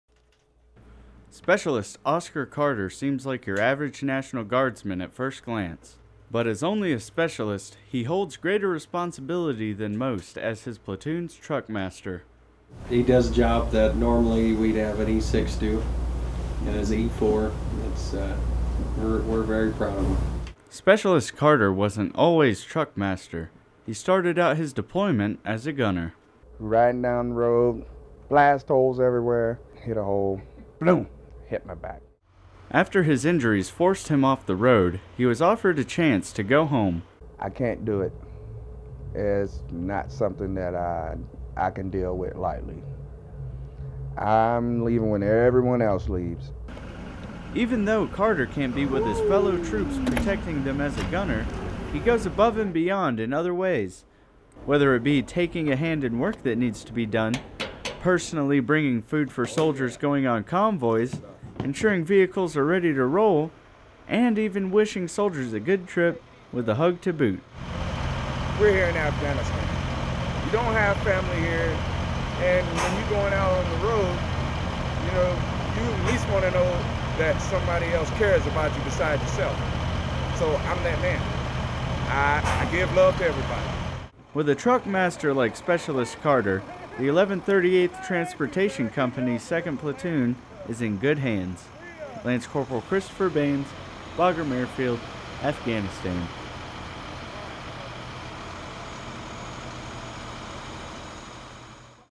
Citizen Soldier Goes Above and Beyond
Throughout history soldiers have often gone above and beyond the call of duty in every facet of the military. LCpl Christofer Baines tells us how one citizen soldier continues the proud tradition in his own way.